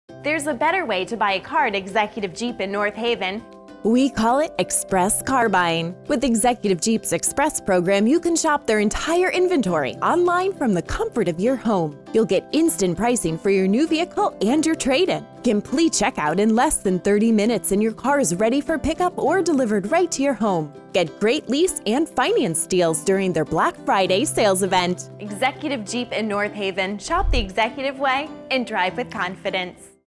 Executive Auto Group: Executive Jeep (BFE Nov)
There's a better way to buy a car at Executive Jeep in North Haven. It's called Express car Buying! Get great lease and finance deals during their Black Friday sales event.